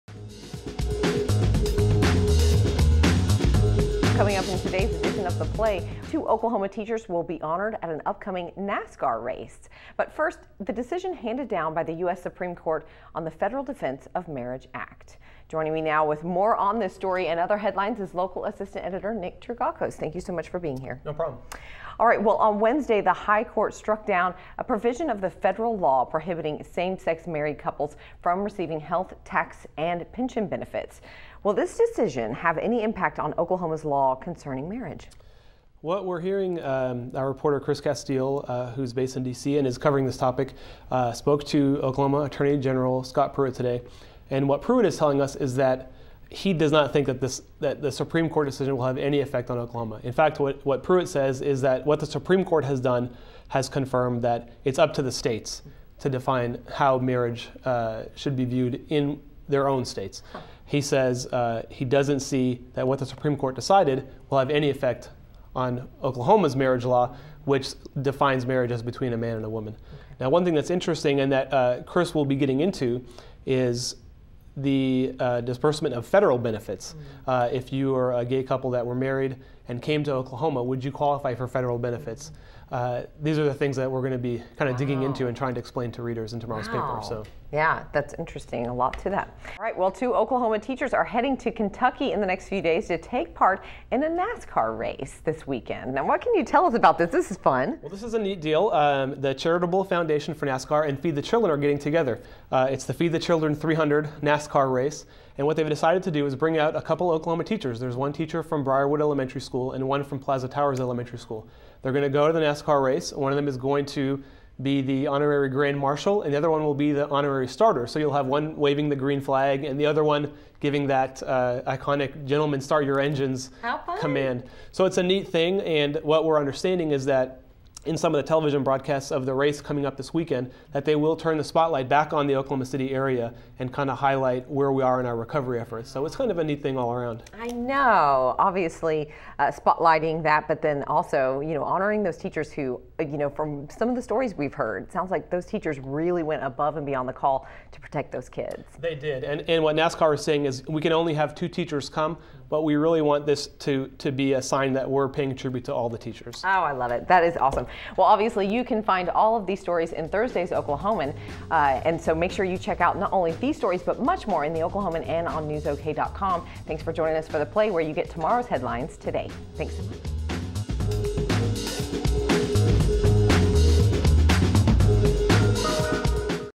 The Play: June 26, 2013
Coming up in Thursday's edition of The Oklahoman: Will the U.S. Supreme Court decisions on gay marriage impact Oklahoma? And two Oklahoma teachers will be honored this weekend at a NASCAR race.